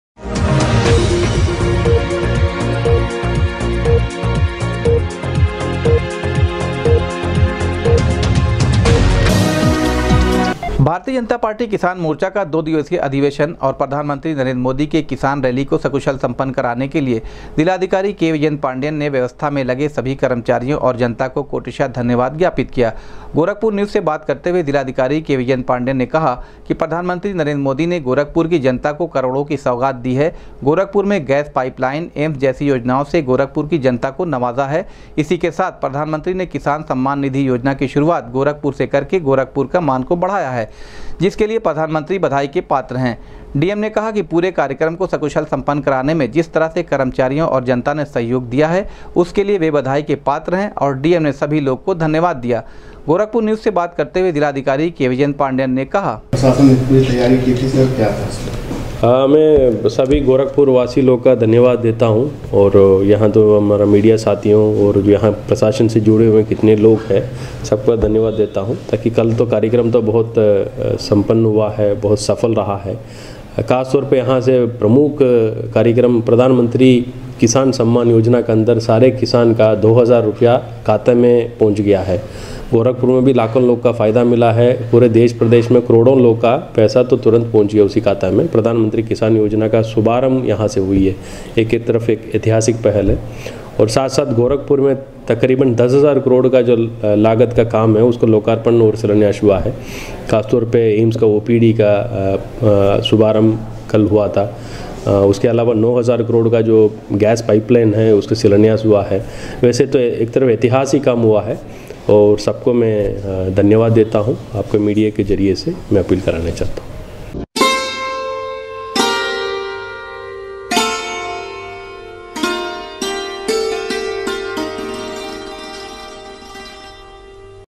किसान रैली के सकुशल सम्पन्न कराने पर जिलाधिकारी ने सभी कर्मचारियों और जनता को
भाजपा किसान मोर्चा के अधिवेशन और किसान रैली के सकुशल सम्पन्न कराने पर जिलाधिकारी ने सभी कर्मचारियों और जनता को दिया धन्यवाद,कहा पीएम ने करोड़ो की योजनाओ के सौगात देकर बढ़ाया जिले का मान।